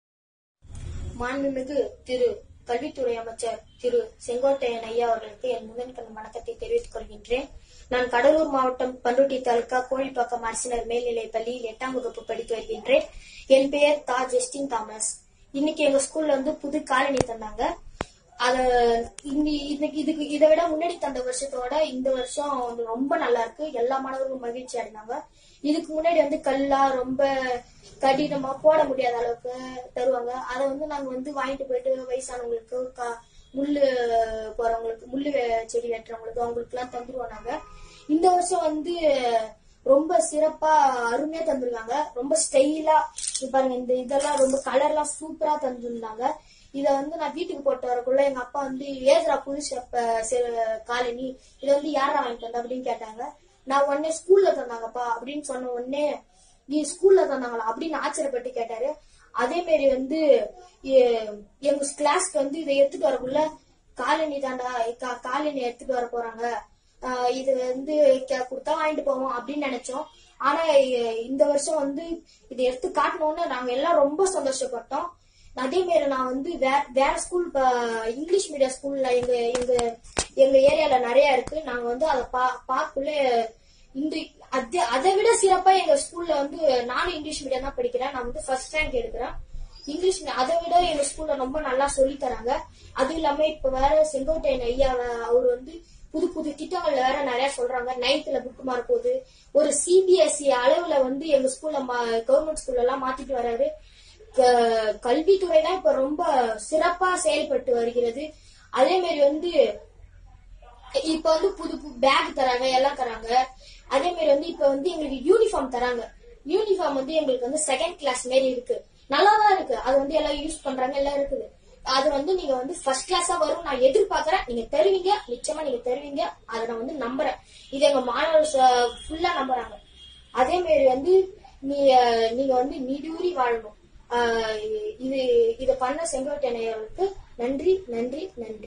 |CUDDALORE| செருப்புடன் அமைச்சருக்கு நன்றி சொன்ன மாணவன்|
அமைச்சர் செங்கோட்டையன் அவர்களுக்கு நன்றி சொன்ன மாணவன்